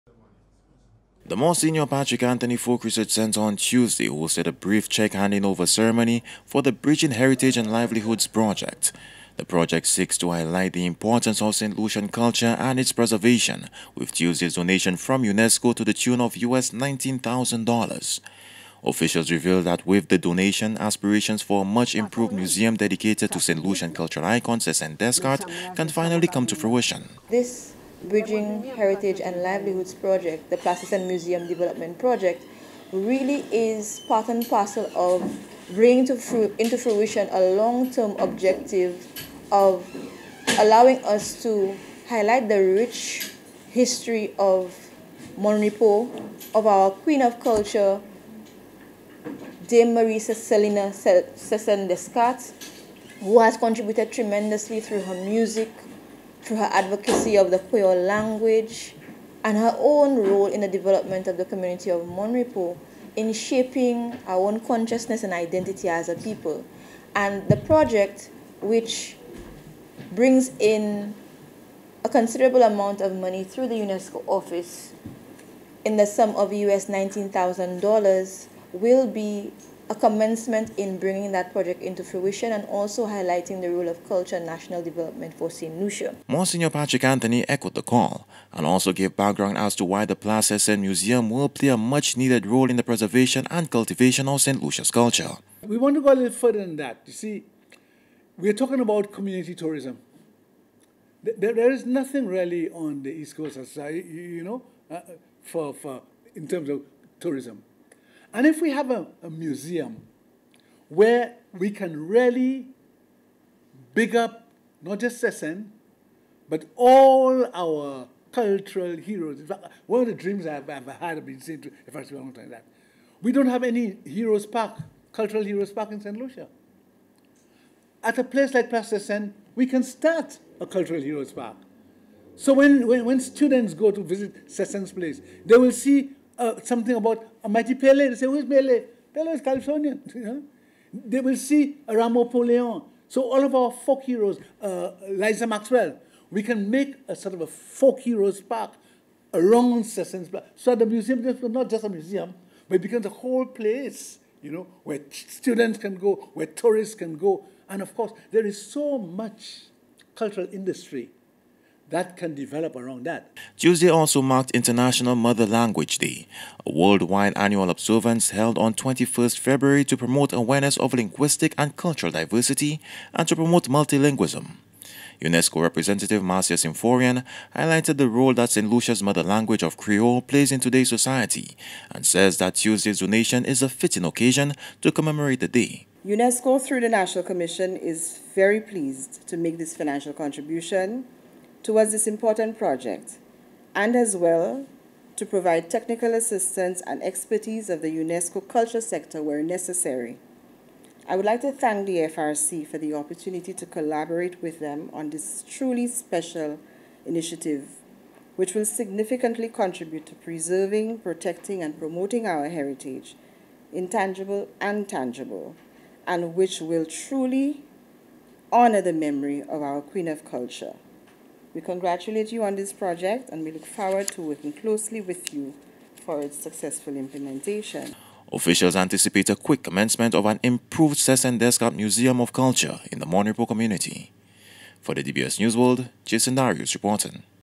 FRC Gets Donation To Help With Sesenne Descartes Museum
The Folk Research Centre on Tuesday received a US nineteen thousand dollar donation to assist with the development of the Sesenne Descartes Culture Museum in Mon Repos.